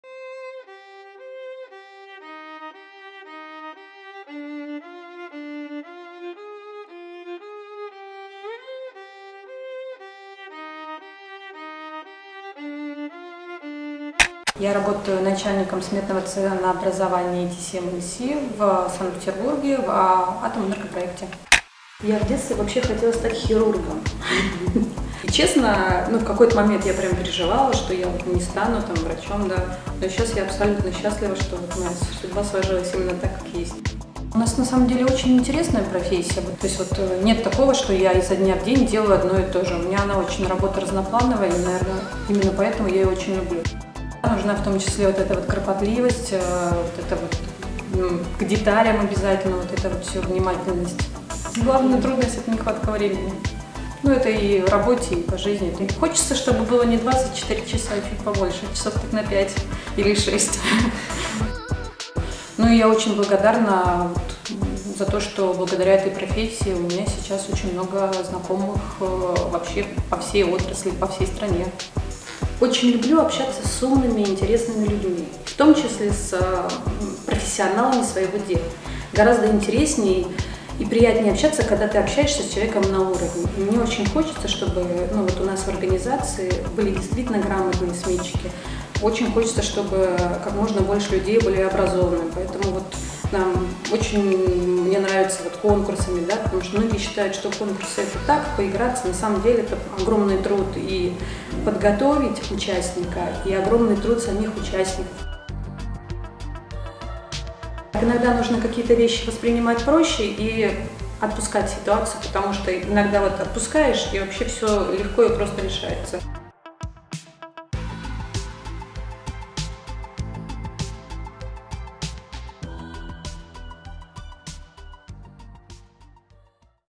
Женщины в атомной отрасли: сметчица
«Я — женщина, а, значит, я…» – проект, посвященный прекрасной половине Инжинирингового дивизиона — необычным, душевным и просто уникальным девушкам.

Лидия Белоус, начальник отдела сметного ценообразования и TCMNC, рассказывает о своей работе, тонкостях отраслевых конкурсов и не только...